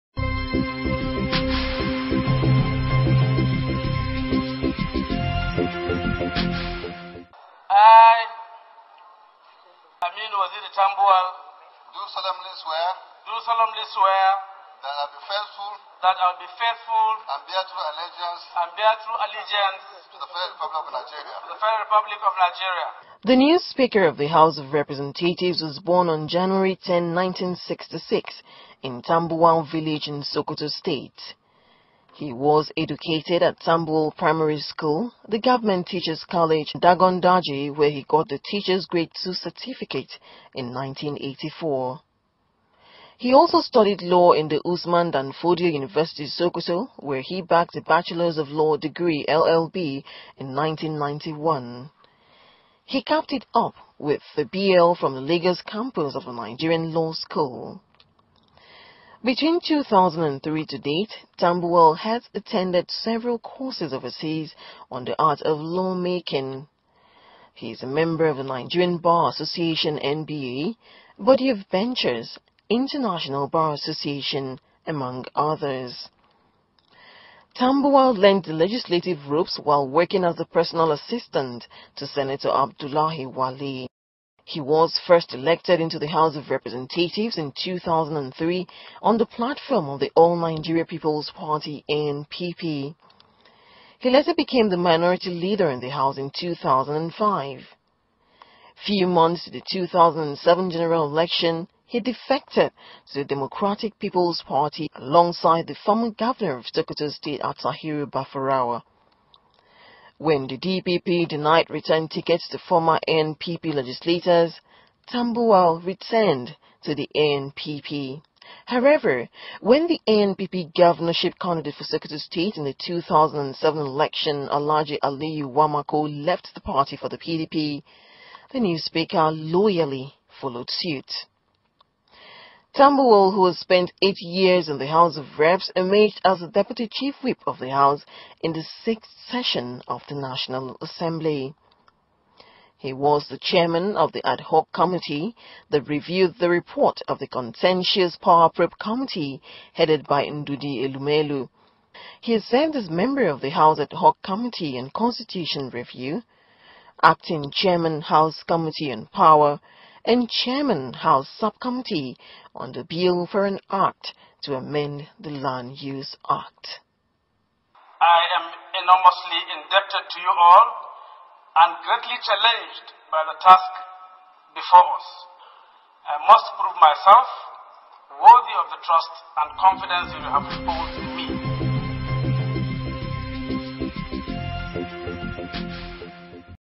Speaker of House of Representatives, Nigeria
Aminu Waziri Tambuwal (born January 10, 1968) is the Speaker of the House of Representatives of Nigeria. He is a People's Democratic Party (PDP) member and represents the Kebbe/Tambuwal Federal Constituency of Sokoto State.

On 6 June, Hon Aminu Tambuwal, 45, emerged as the new Speaker of the House of Representatives, at the inauguration of the seventh session of the Federal legislature in Abuja.

He was elected by a simple majority vote of 252 out of the 340 members of the House, beating his opponent, Rep. Mulikat Akande-Adeola (People's Democratic Party-Oyo) who scored 90 votes (There were 8 invalid votes and one member abstained).